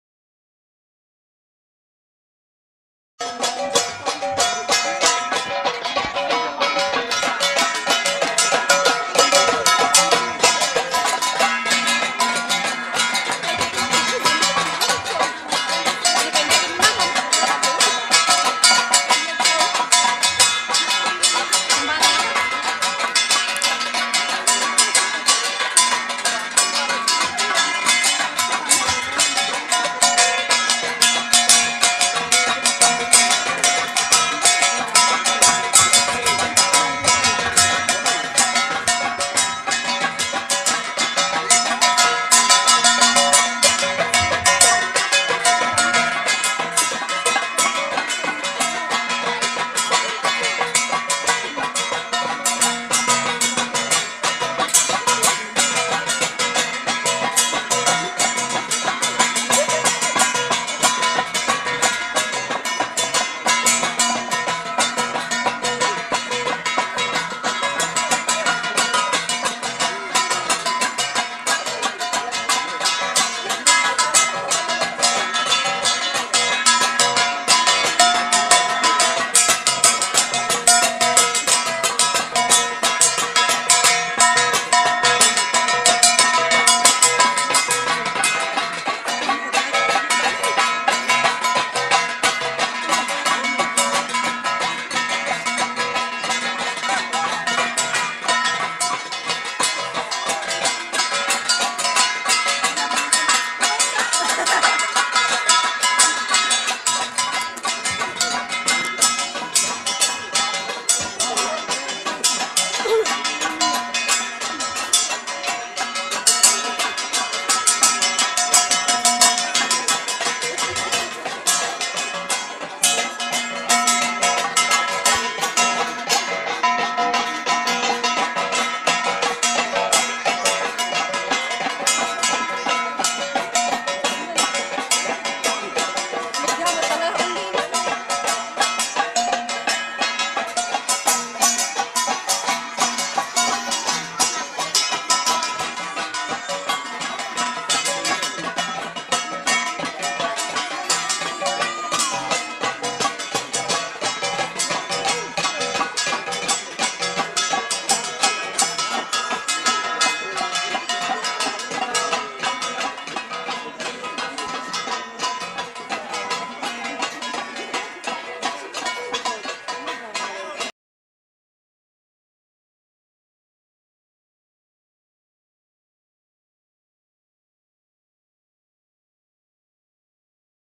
Eagle Dance - with "Wanes"(G String)("Bahag")
The music and videos are free to use, however, please mention or give credits to my Youtube Channel

This video was taken last 2014 at La Trinidad Benguet.